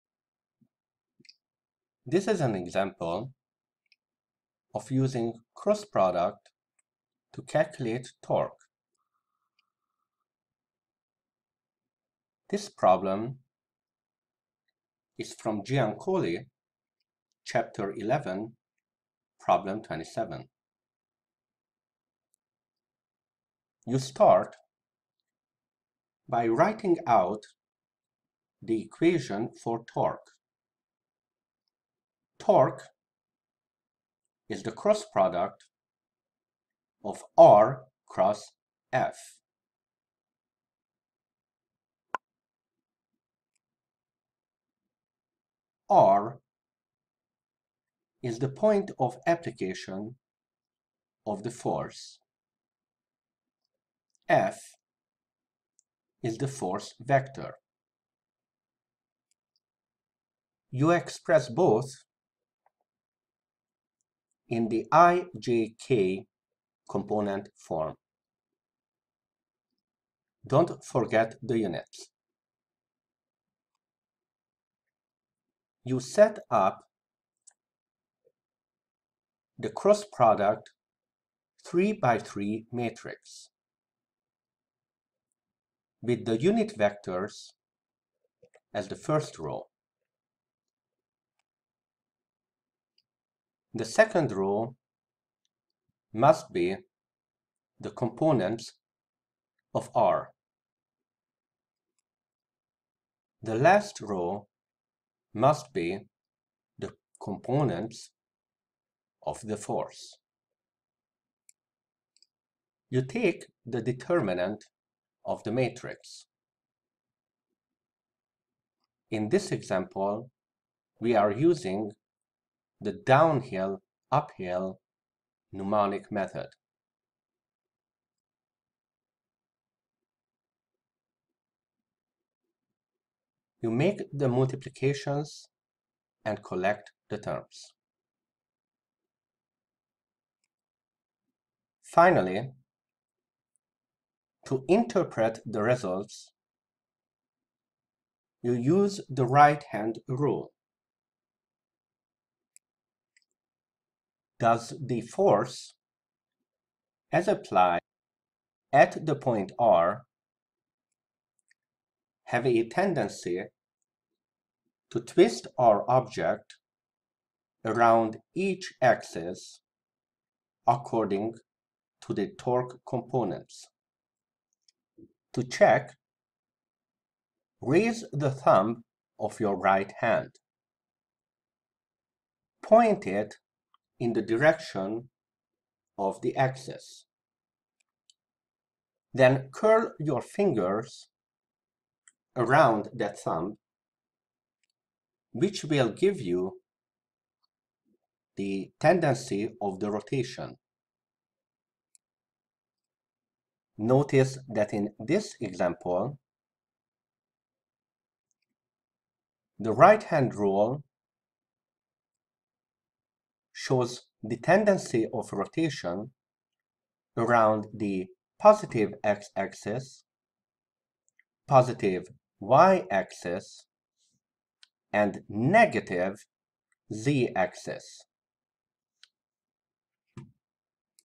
Cross product: use for torque
Example problem calculating torque, using the matrix treatment of vector cross-product.